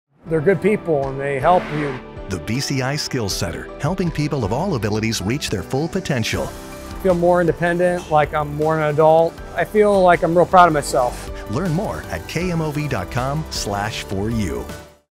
Meet Shane! A BCI Skills Center Graduate
Shane Morrissey is a BCI Skills Center Graduate.